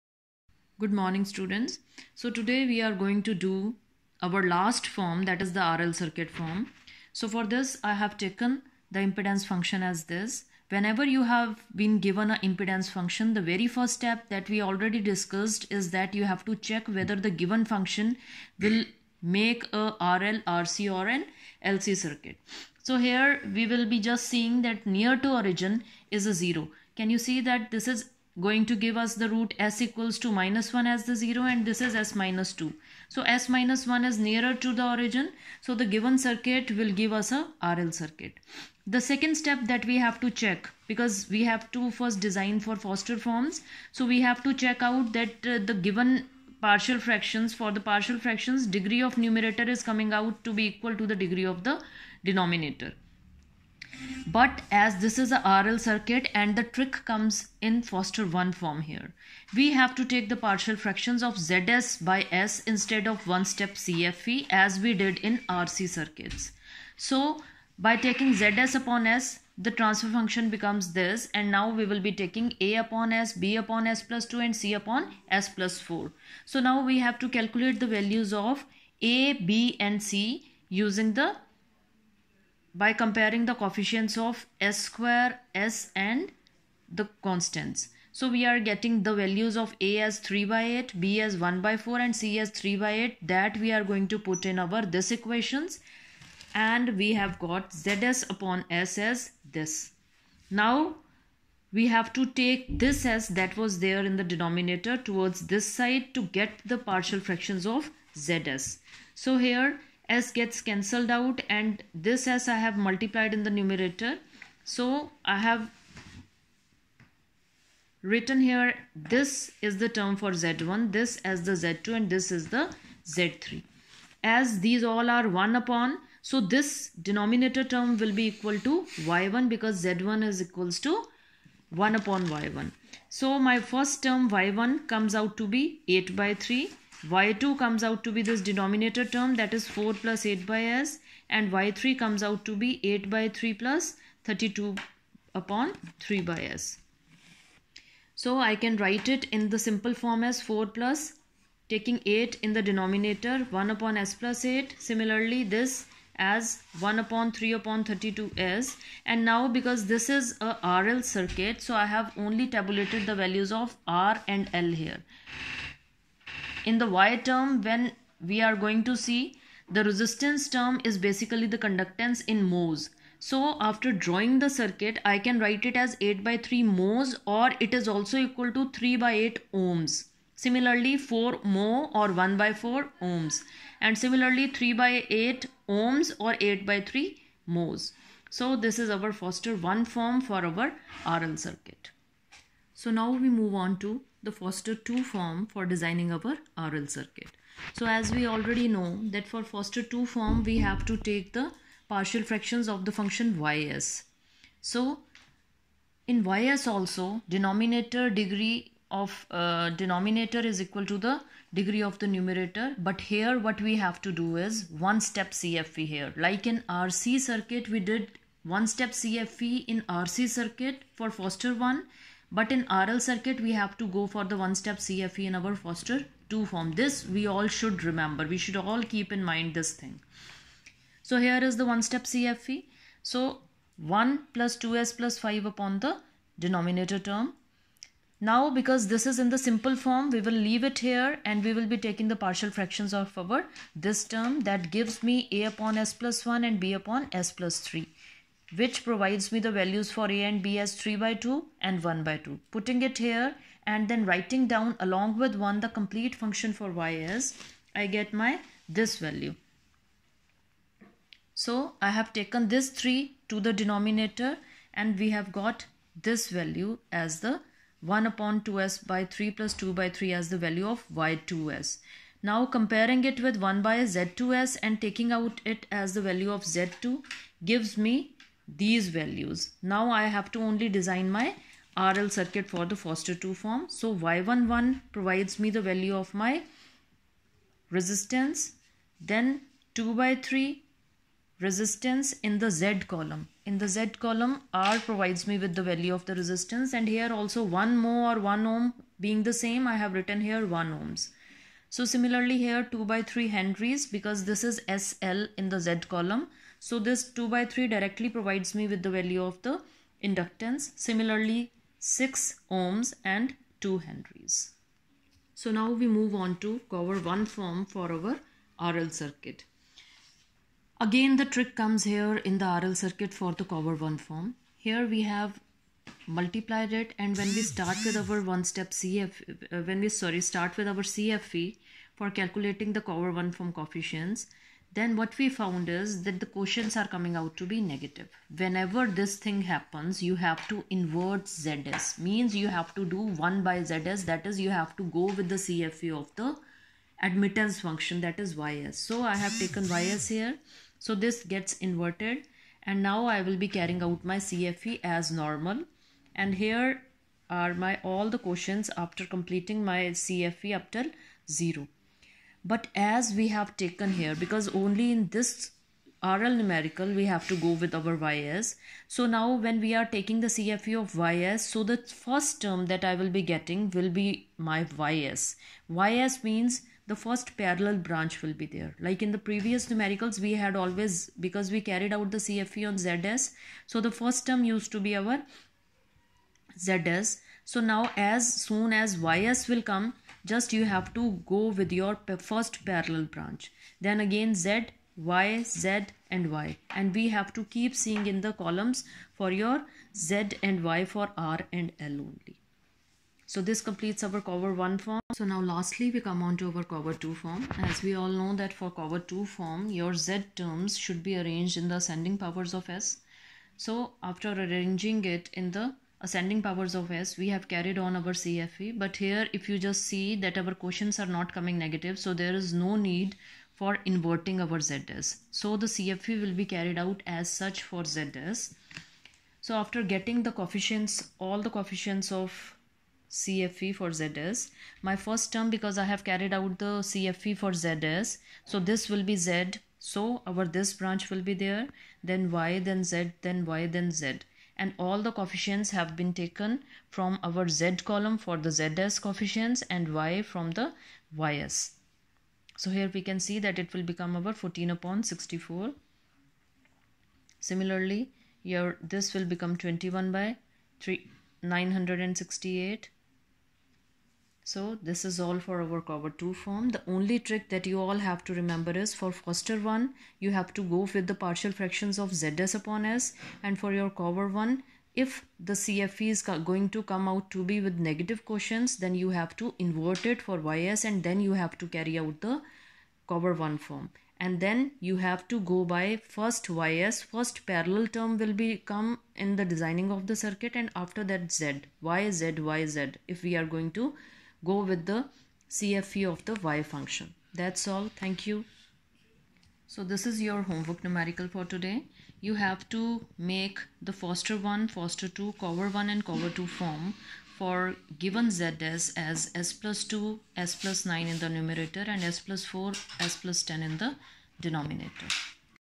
Understanding trick for RL circuit Foster and Cauer forms
related to this lecture the students can learn foster form trick for RC circuits and cauer forms for RC circuits as well as foster 1 and 2 for LC circuits and Cauer 1 and 2 for LC circuits. the links to the same are
To Master yourself in NEtwork Synthesis numericals and control concepts.